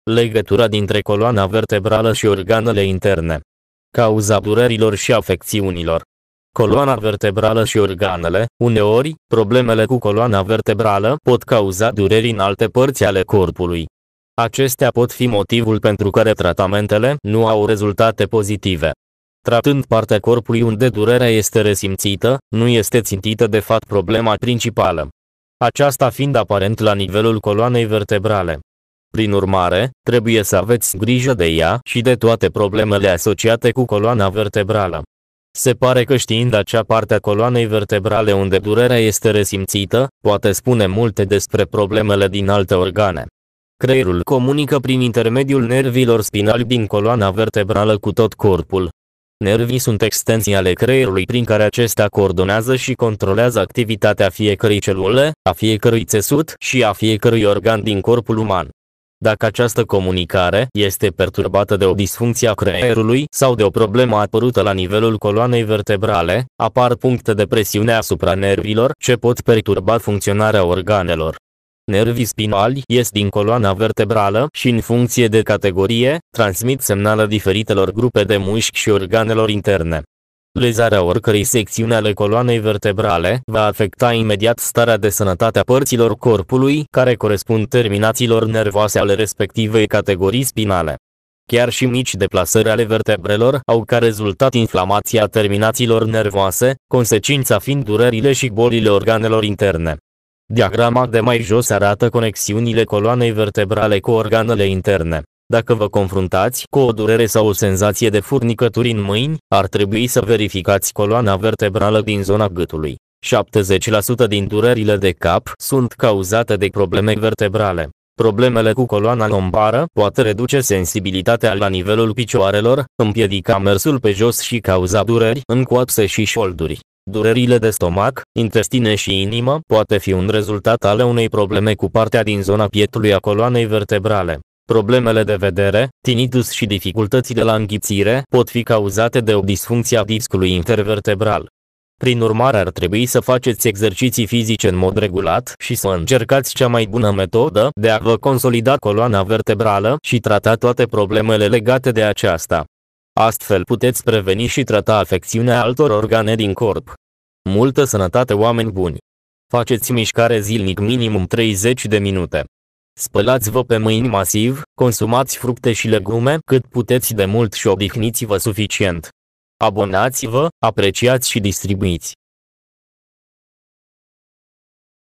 Legătura dintre coloana vertebrală și organele interne. Cauza durerilor și afecțiunilor!
Coloana vertebrală și organele – Uneori, problemele cu coloana vertebrală pot cauza dureri în alte părţi ale corpului. Acestea pot fi motivul pentru care tratamentele nu au rezultate pozitive. Tratând partea corpului unde durerea este resimţită, nu este ţintită de fapt problema principală.
Dacă doriți s-aflați care este legătura dintre coloana vertebrală și organele interne, rămâneți alături de noi până la final! Abonați-vă, apreciați și distribuiți!